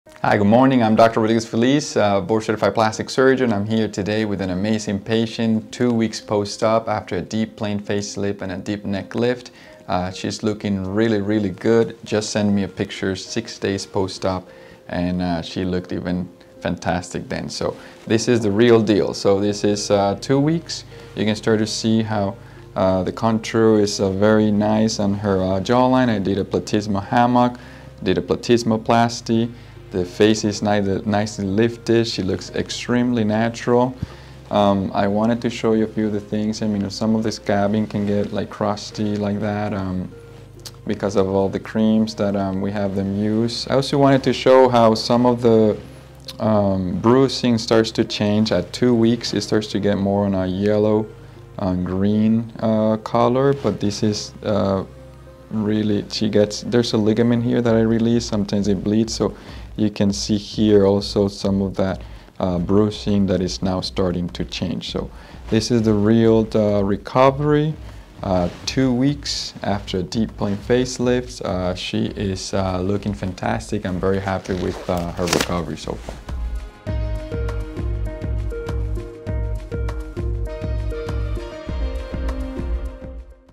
How would I look 2 weeks after a facelift?
I believe that the most important thing for a patient is not only the final result of their surgery but also the process to get there.
That's why I like to show you what patients look like at each stage of recovery, so you are informed and ready for this journey.
All content is posted with permission and is the property of Solestica.